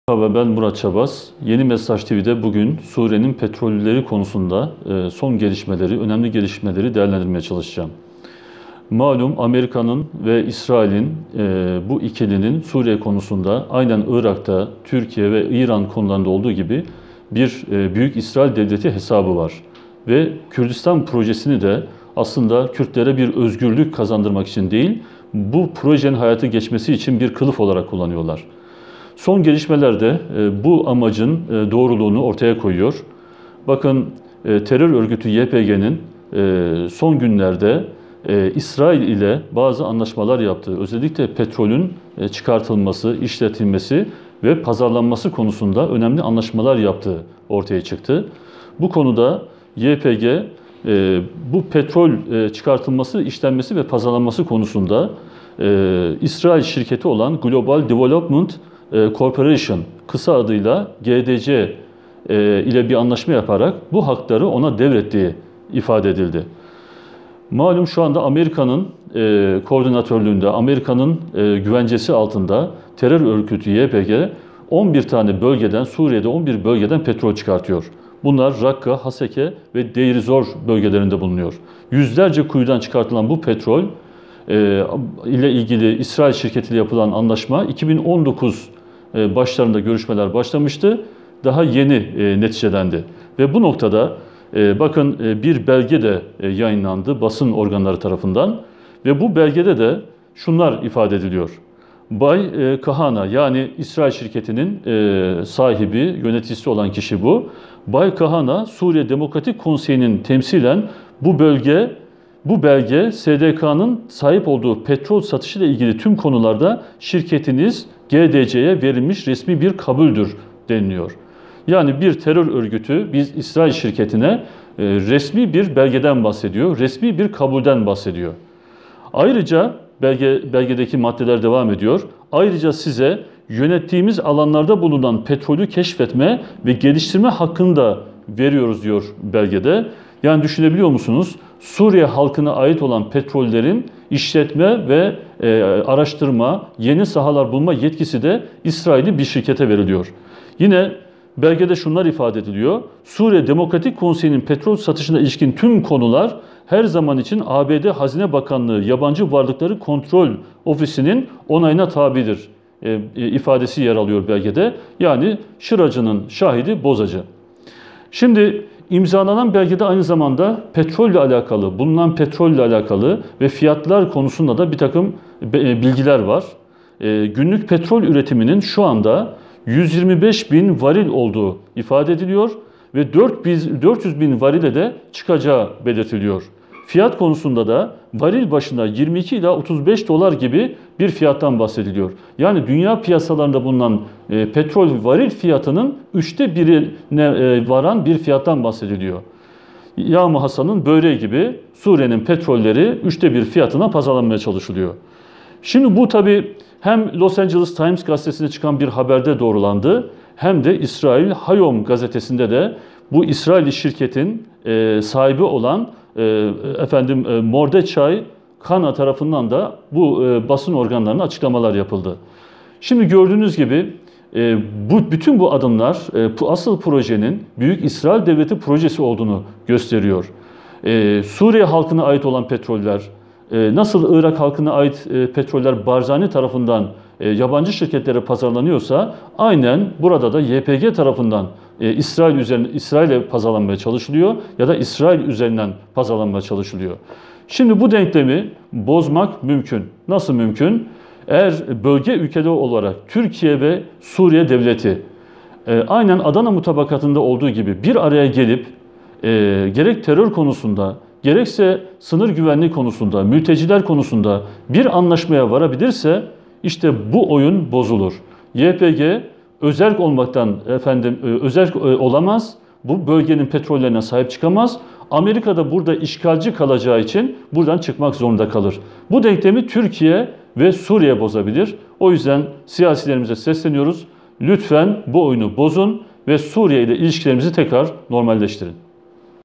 SURİYE'NİN PETROLÜNE İSRAİL ÇÖKTÜ
ABD ile İsrail'in Ortadoğu'daki asıl hedefleri "büyük İsrail projesi" olduğu artık sır değil.
Yaşanan son gelişmeler de bu iki devletin şer ittifakının bu hedefe doğru ilerlediğini gösteriyor.
ABD önce Suriye'den çekileceğini ilan edip, ardından petrol bölgelerinde askeri varlığını devam ettireceğini açıklamıştı ve bu bölgelere yeni yeni üsler kurmaya başlamıştı. Rusya, bu gelişmeler üzerine ABD'yi Suriye'nin petrollerini çalmakla suçlamıştı.
Bu suçlamalar üzerine ABD, petrolü YPG'nin oluşturduğu Suriye Demokratik Güçleri'nin (SDG) çıkartıp, gelirlerini kullandığını açıklamıştı.
Gelen bilgilere göre YPG, petrolün tüm işletme haklarını İsrailli Global Development Corporation'a (GDC) devretti; İsrailli bu şirket hem petrol sahalarını işletecek, hem de çıkan petrolü satacak.

Yasal Uyarı
Yorum kuralları: